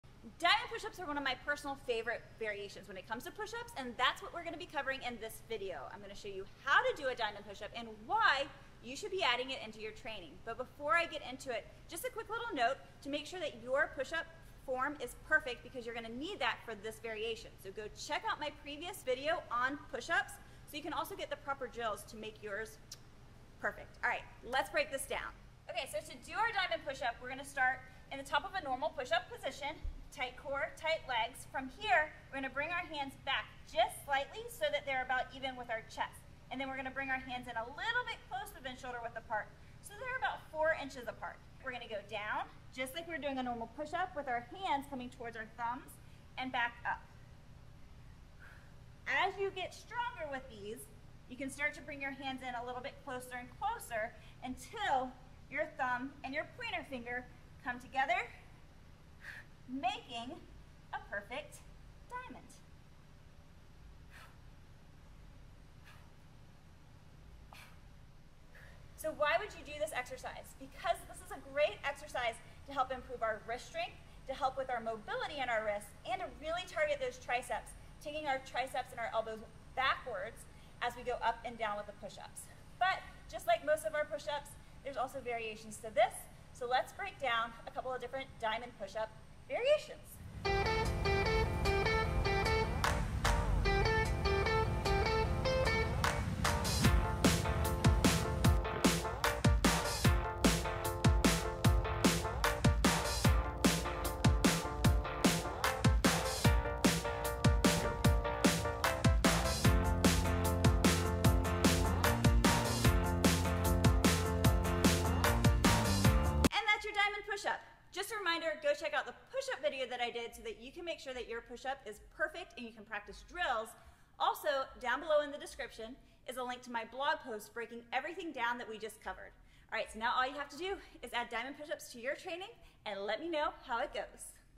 How To Do Diamond Pushups - Exercise Database
Diamond Pushups: Why do them and how to do diamond pushups properly.

What's your favorite variation?

➡️ Works on wrist strength and mobility
➡️ Extra emphasis on the triceps

And yes, they are a fun way to change things up too!

Get the full breakdown and variations on the blog